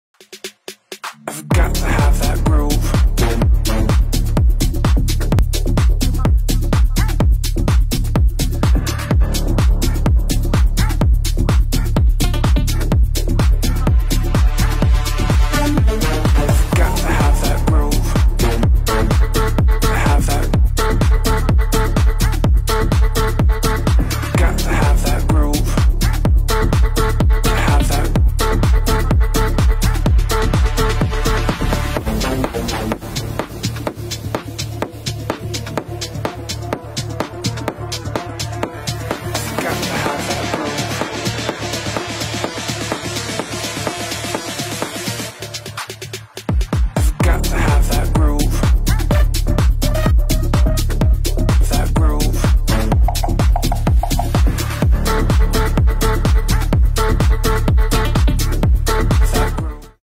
Il primo giorno di Rosamaria Montibeller alla e-work Busto Arsizio - UYBA
💥 Il primo giorno di Rosamaria Montibeller, argento mondiale con il Brasile, alla e-work Busto Arsizio!